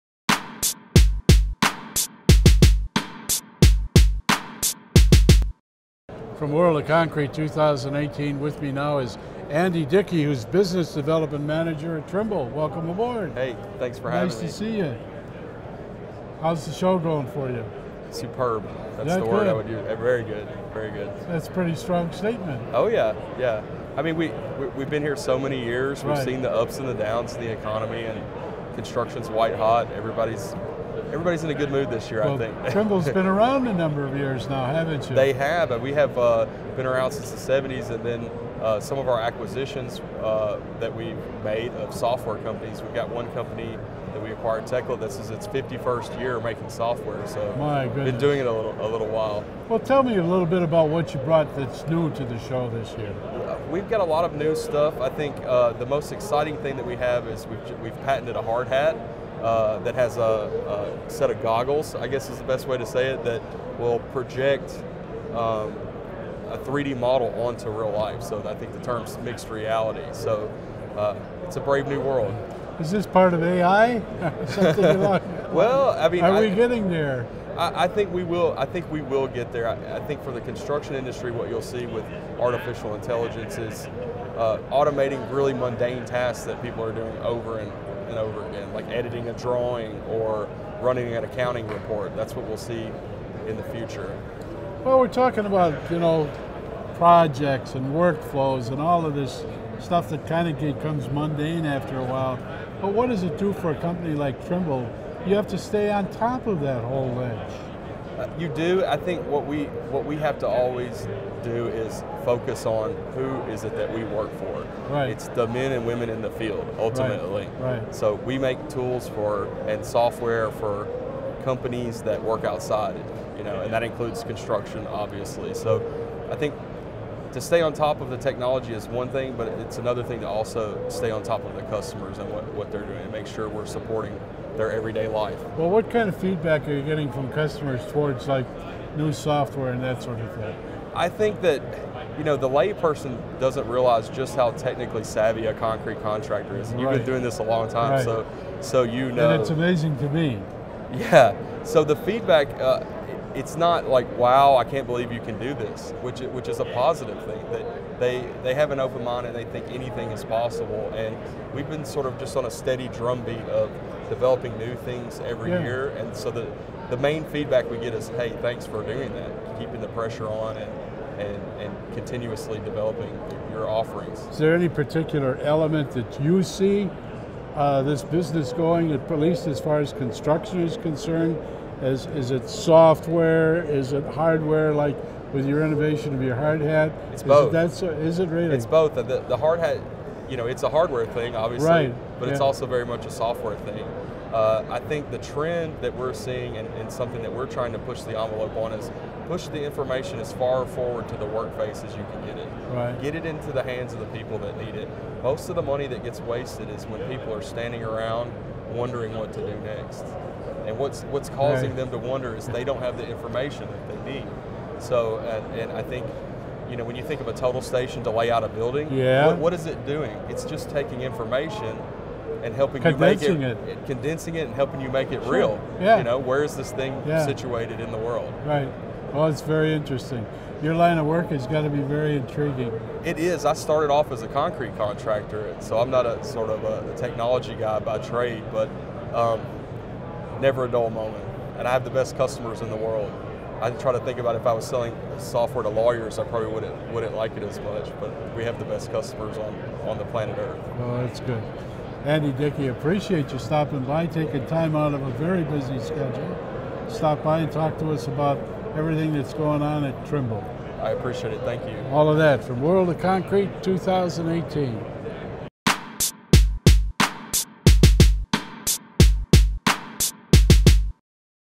Trimble Patents New Goggles that Project 3D onto Real World
Hardhat with goggles will project a 3D model onto real life